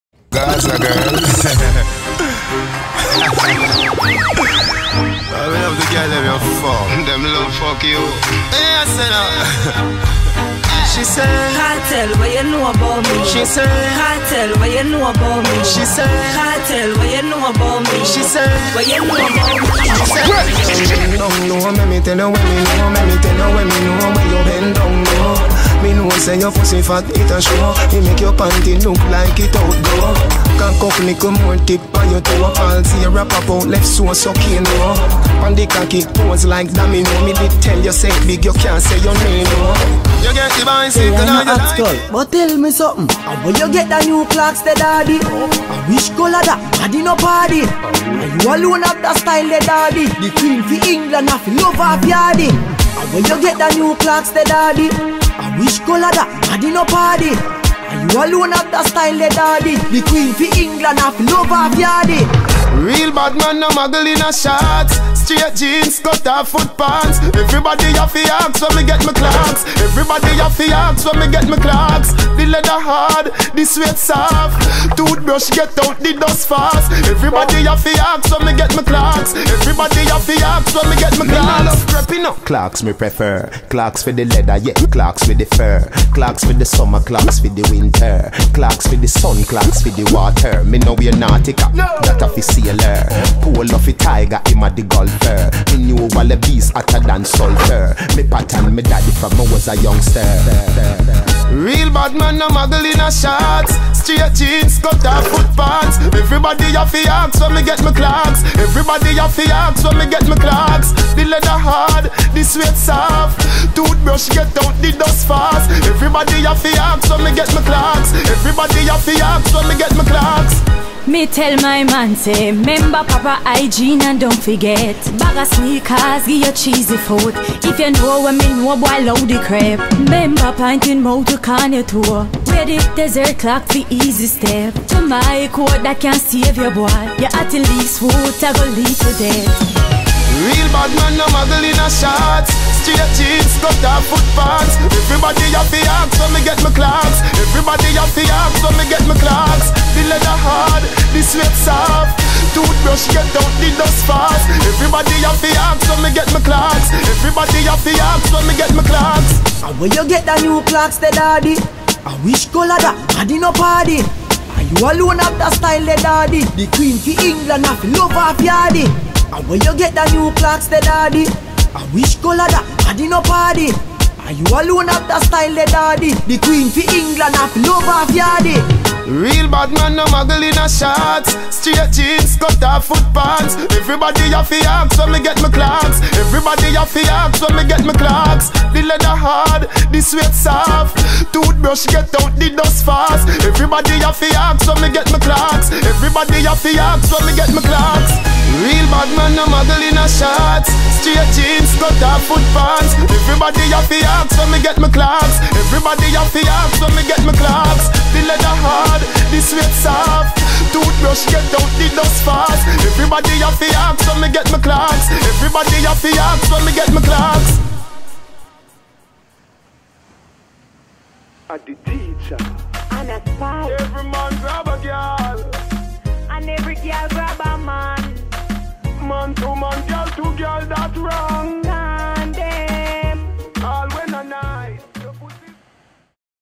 Film Timothé Bisso Gwada Rider.m4v
Film de surf de Tim Bisso 14 ans